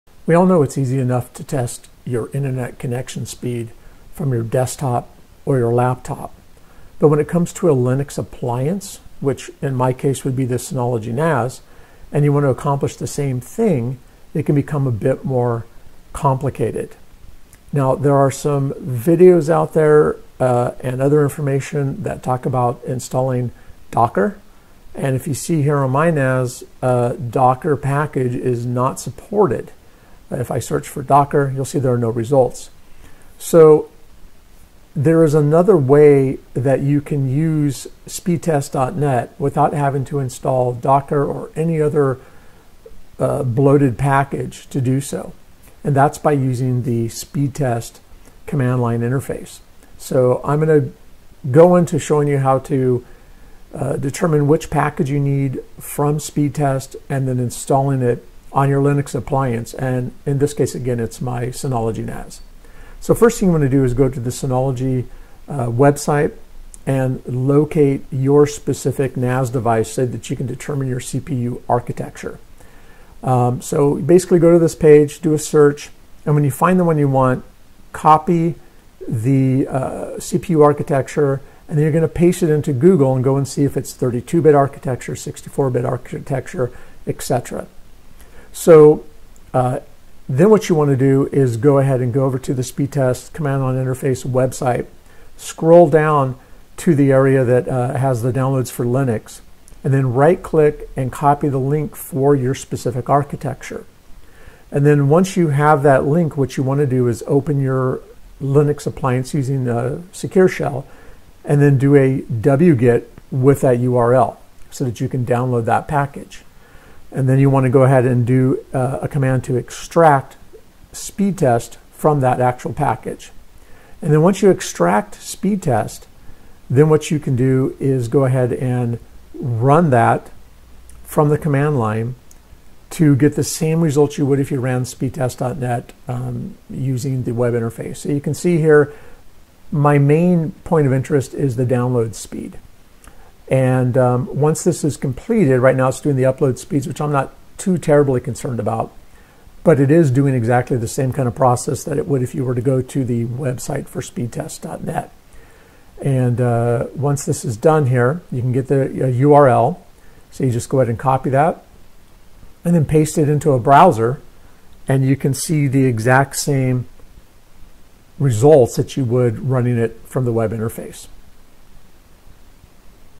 Synology NAS Internet Speed Test
The Speedtest.net command line interface (CLI) can be used to determine the internet speed of a Linux appliance.